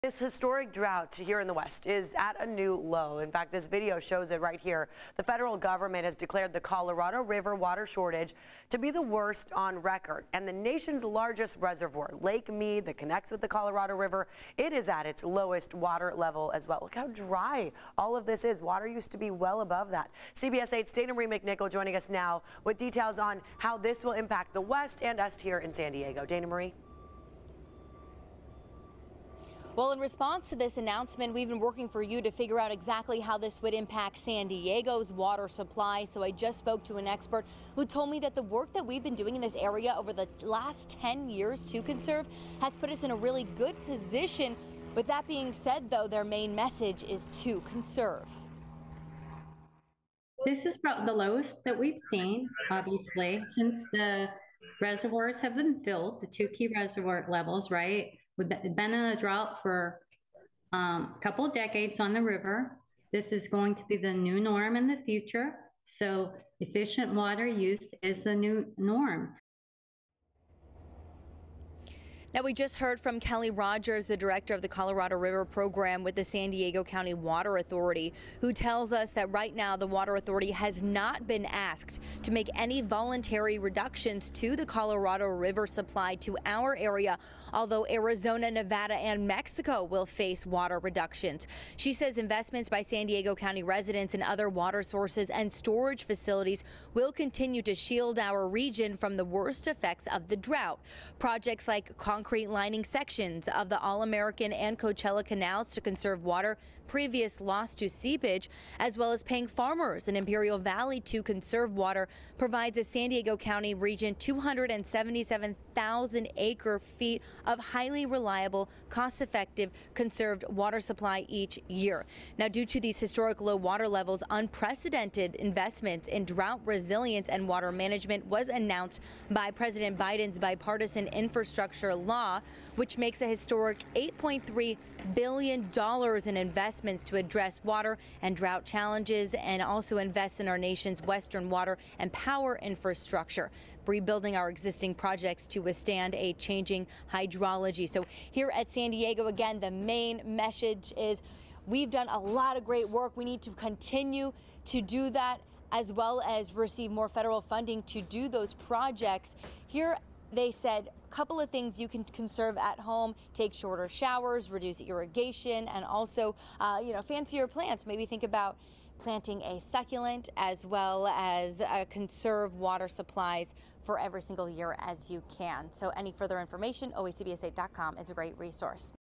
Water conservation is critical in San Diego County as Colorado River declines
Scientists say the U.S. West, including the Colorado River basin, has become warmer and drier in the past 30 years. MORE